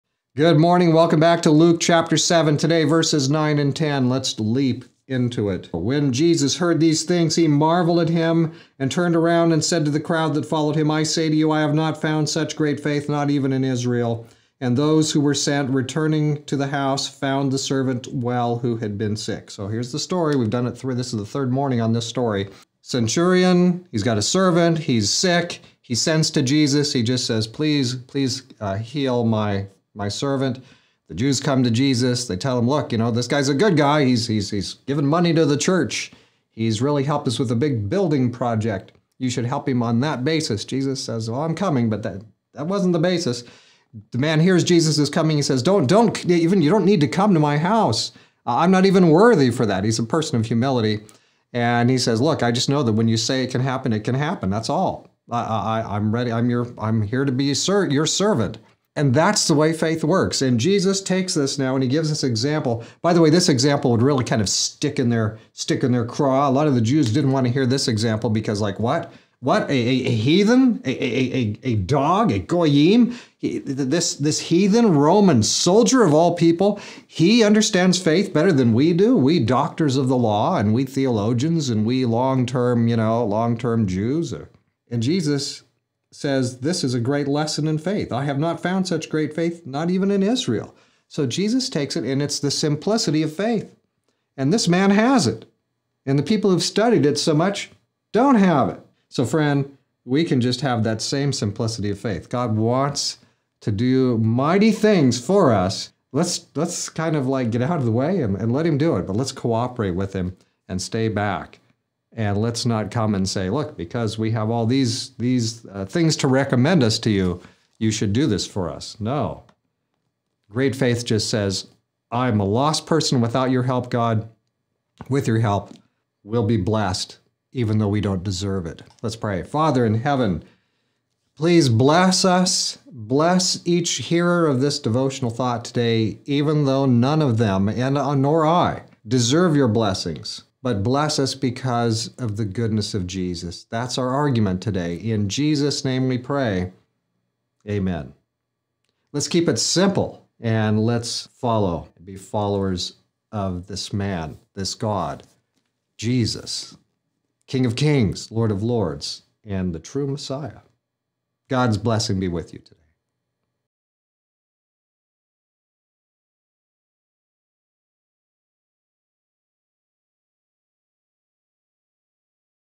Luke 7:9-10 Faith Superior
In the centurion Jesus has a simple and powerful lesson in faith. Not the learned and pious long-term teachers of the law but the straightforward faith of an unlearned soldier takes the day.